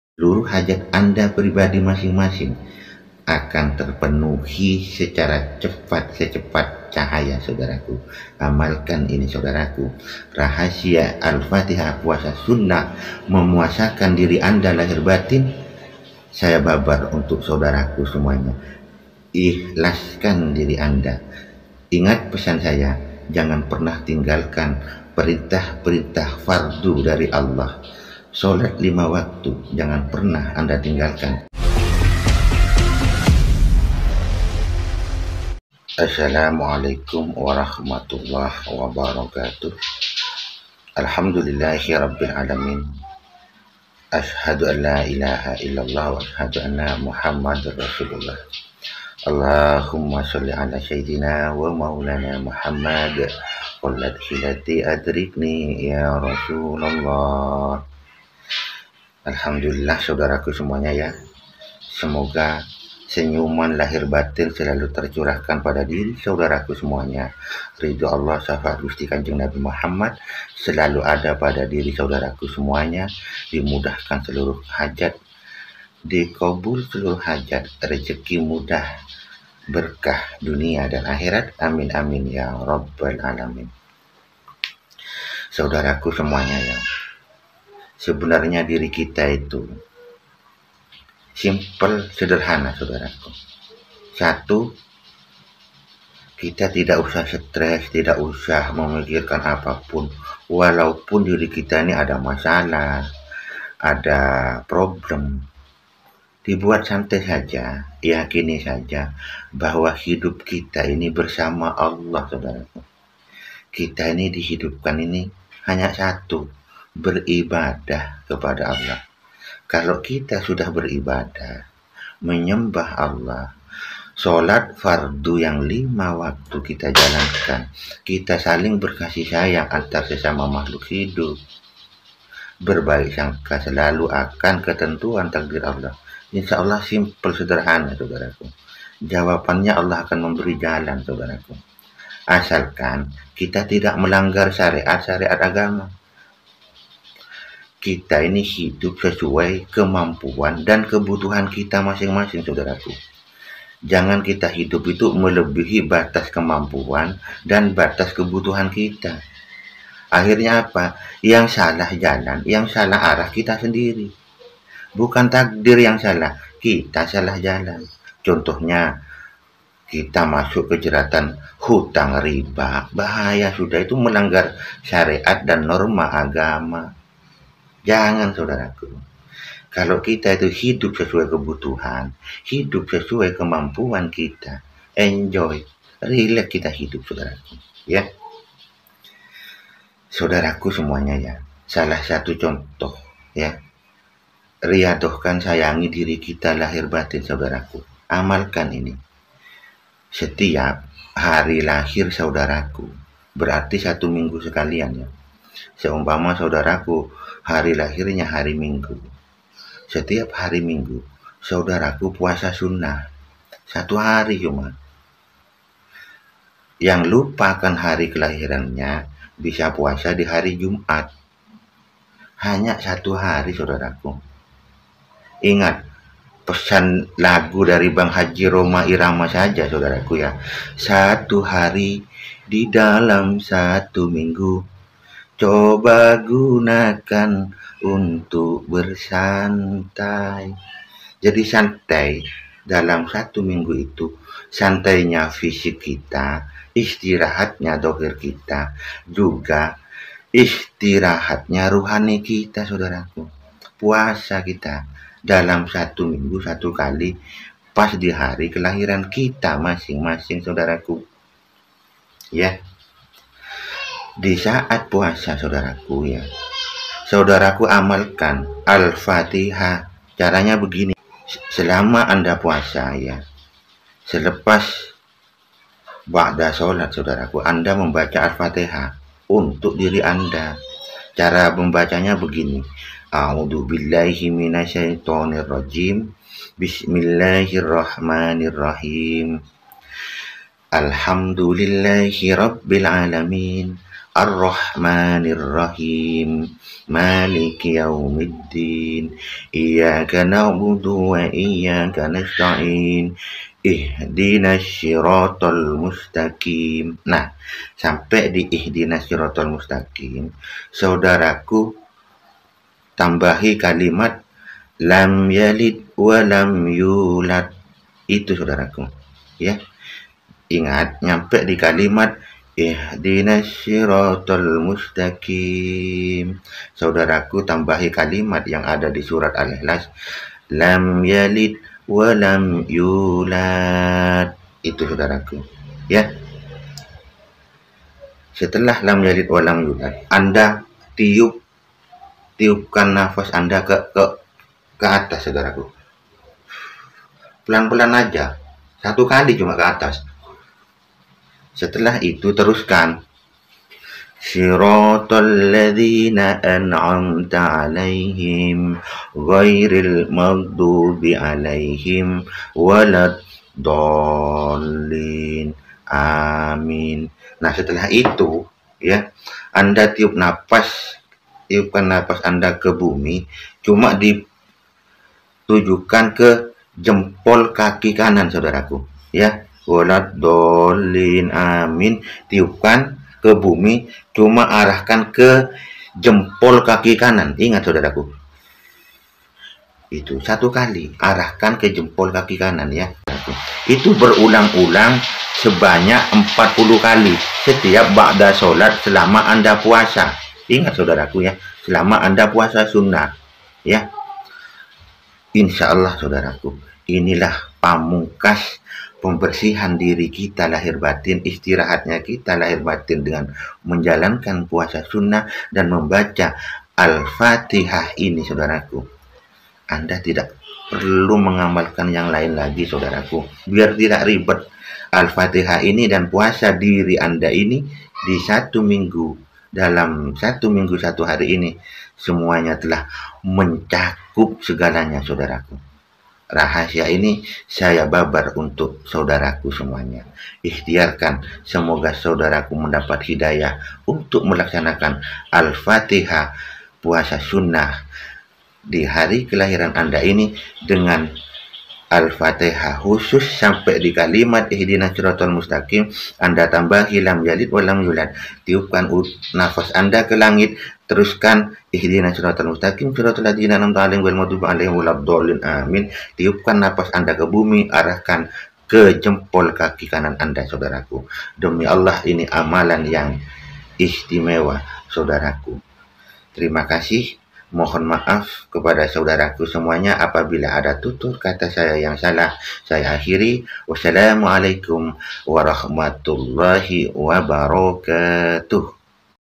PUASA DIRI DAN AL FATIHAH "LAM YALID WA LAM YULAD" AMALAN BATIN YG HARUS KITA AMALKAN
PUASAKAN DIRI KITA 1 HARI DI SETIAP HARI LAHIR KITA DAN AMALKAN AL FATIHAH INI 40 KALI SETIAP SELESAI SHOLAT FARDHU ,
INI AKAN MEMBANTU MEMPERCEPAT KEINGINAN HAJAT KITA .